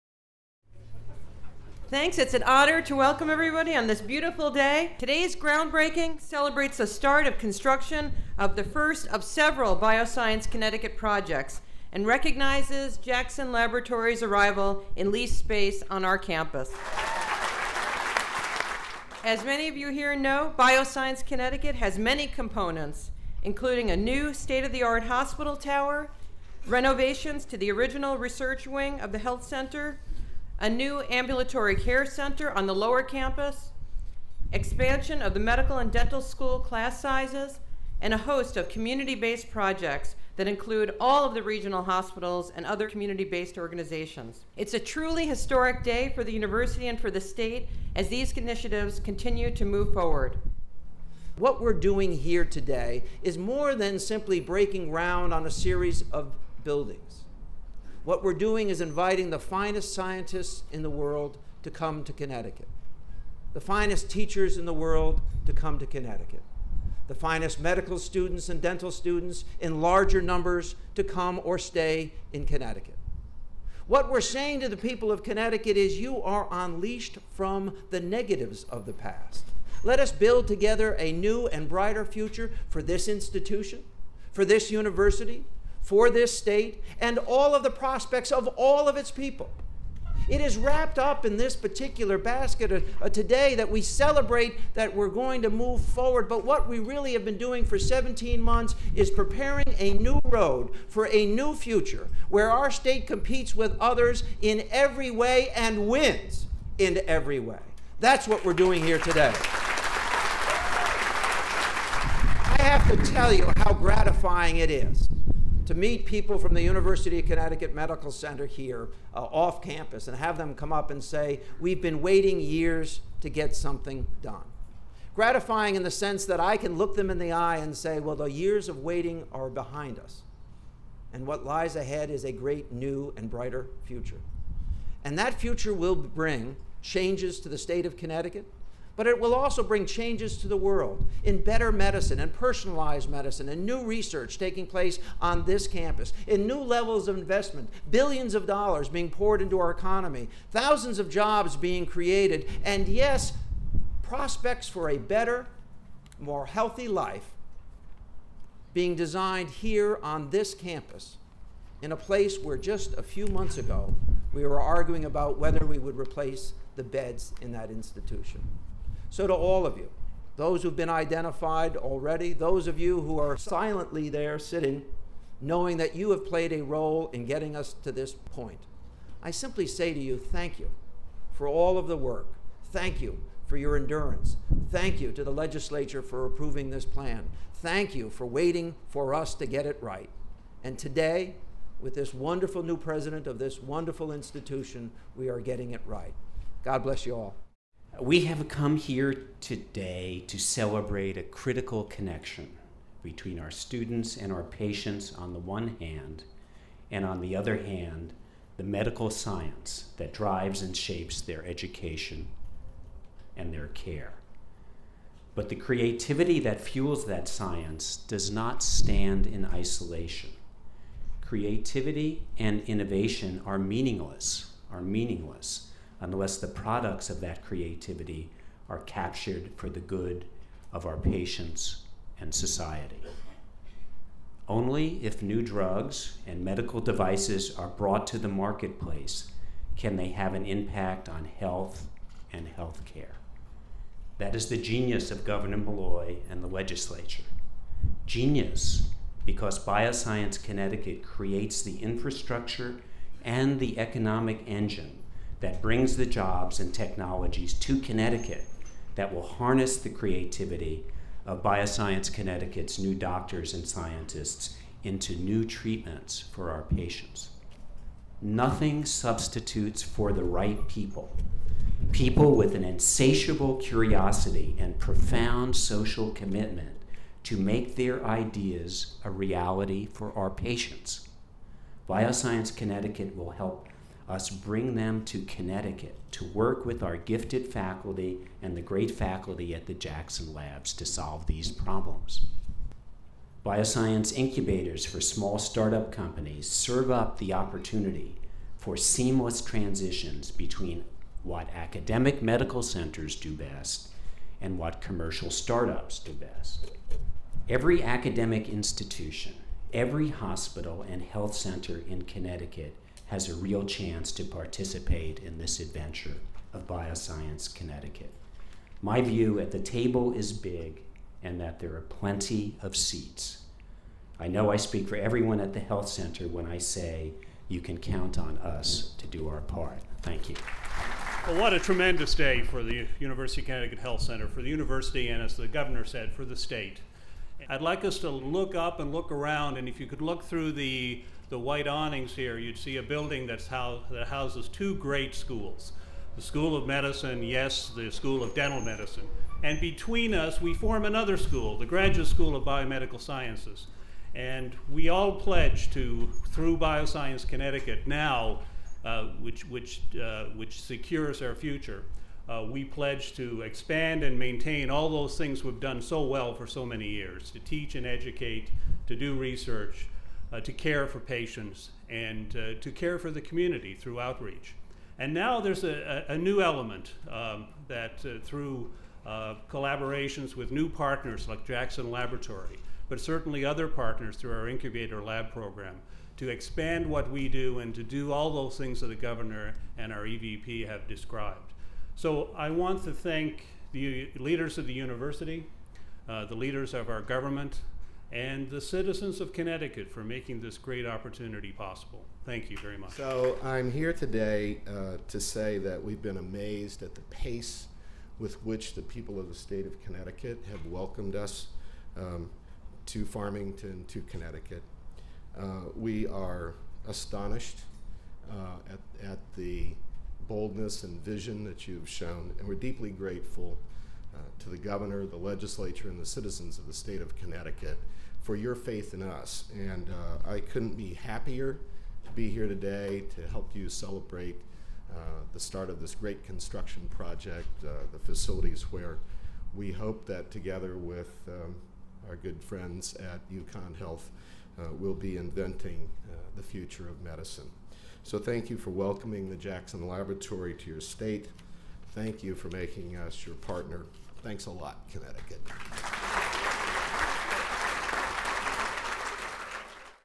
Bioscience Connecticut Groundbreaking Ceremony
Less than a year after he signed the Bioscience Connecticut bill, Gov. Dannel P. Malloy was joined by UConn leaders, state officials, and the UConn Health Center community for a groundbreaking ceremony to mark the start of a series of construction projects on the Health Center campus.